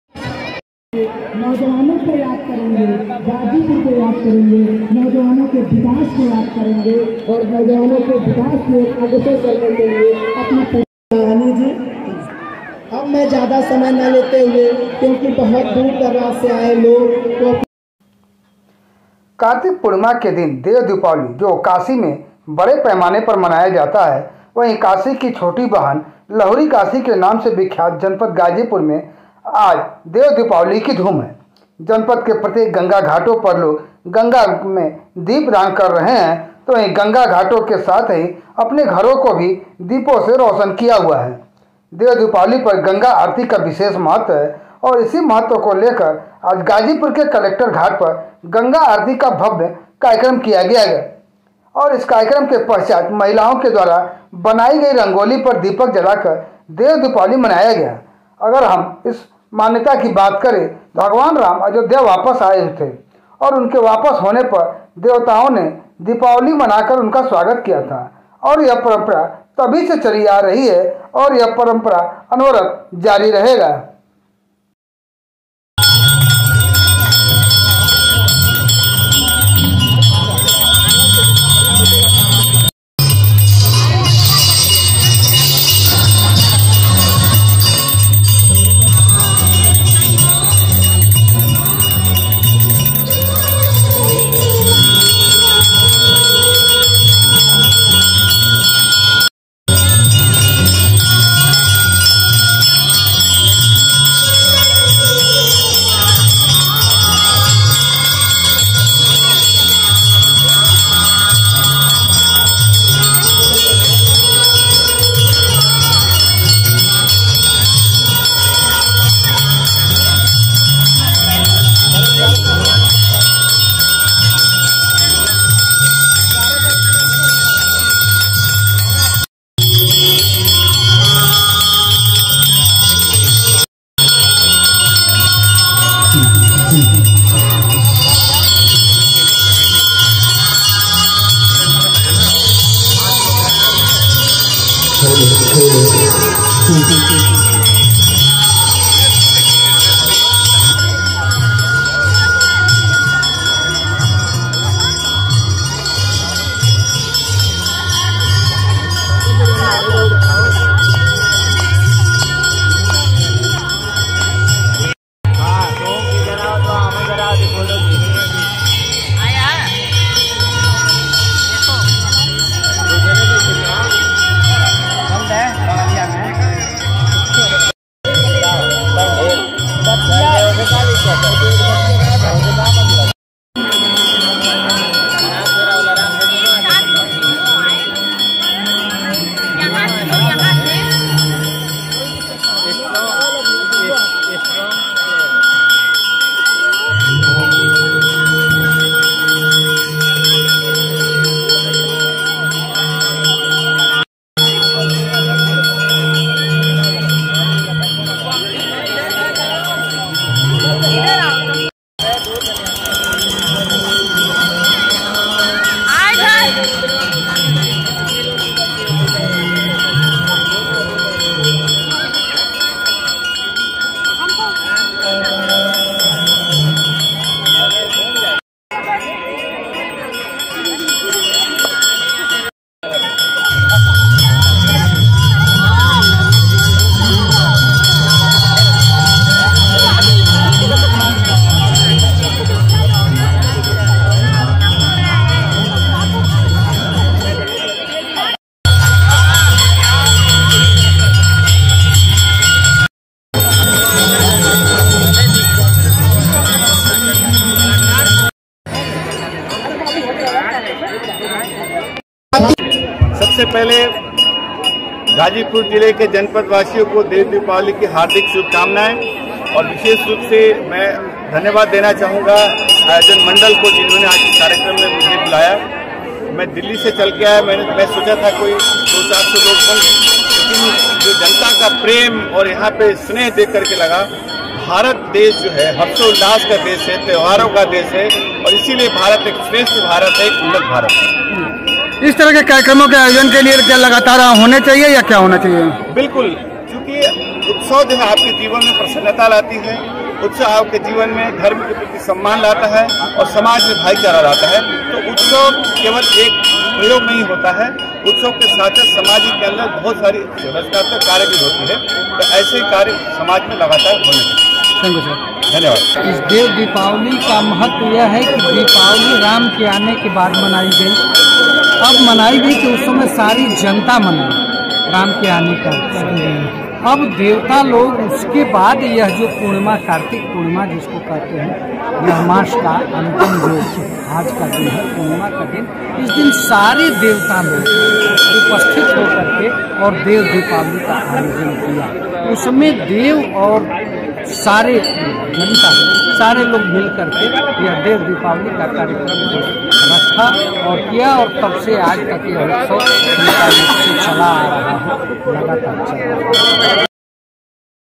लहुरी काशी में देव दीपावली पर गंगा घाट हुआ दीपो से रौशन
MMC NEWS
कार्तिक पूर्णिमा के दिन देव दीपावली जो काशी में बड़े पैमाने पर मनाया जाता है वही काशी की छोटी बहन लहुरी काशी के नाम से विख्यात जनपद गाजीपुर मैं आज देव दीपावली की धूम  है। जनपद के प्रत्येक गंगा घाटों पर लोग गंगा में दीपदान कर रहे हैं तो वही गंगा घाटों के साथ ही अपने घरों को भी दीपो से रोशन किया हुआ है देव दीपावली पर गंगा आरती का विशेष महत्व है और इसी महत्व को लेकर आज गाजीपुर के कलेक्टर घाट पर गंगा आरती का भव्य कार्यक्रम किया गया और इस कार्यक्रम के पश्चात महिलाओं के द्वारा बनाई गई रंगोली पर दीपक जलाकर देव दीपावली मनाया गया अगर हम इस की मान्यता की बात करें तो आज ही के दिन भगवान राम अयोध्या वापस हुए थे और उनके वापस होने पर देवताओं ने दीपावली मना कर उनका स्वागत किया था और यह परंपरा तभी से चली आ रही है और यह परंपरा अनवरत जारी रहेगा।